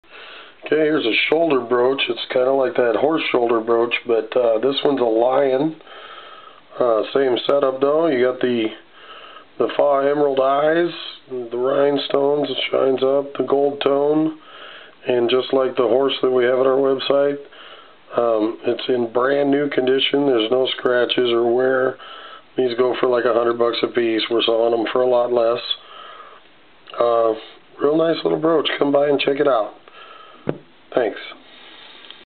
Vintage Gold Tone & Crystal Lion Shoulder Brooch Pin
Gorgeous Rhinestone & Gold King of the Jungle Vintage Shoulder Brooch in mint condition. Wear this stunningly beautiful brooch with your favorite jacket or fur. Will definitely make a statement.
This Extremely large brooch is gold colored metal with tons and tons of clear rhinestones encrusting the entire enameled surface. The piece is in perfect shape.